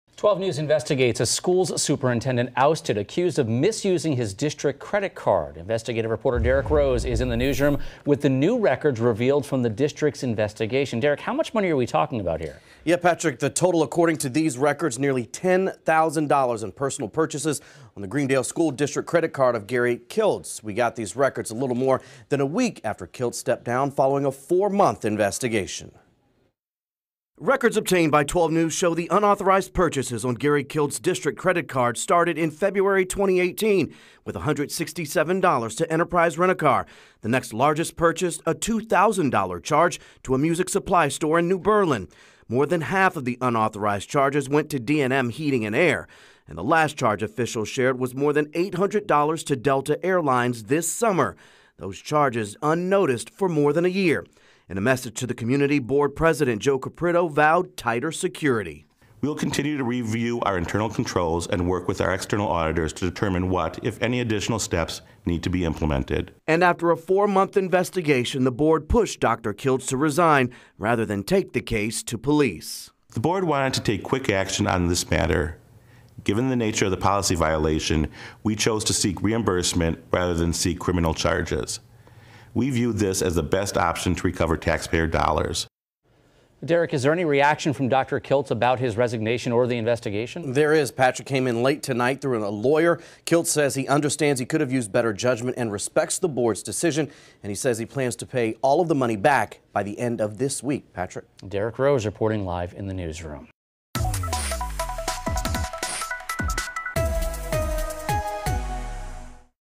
Greendale Superintendent goes on spending spree with district credit card
Greendale Schools Superintendent Dr. Gary Kiltz was forced to resign his position after an investigation revealed Kiltz spent nearly $10,000 on home repairs and family travel, records obtained by the WISN 12 News Investigative Unit show.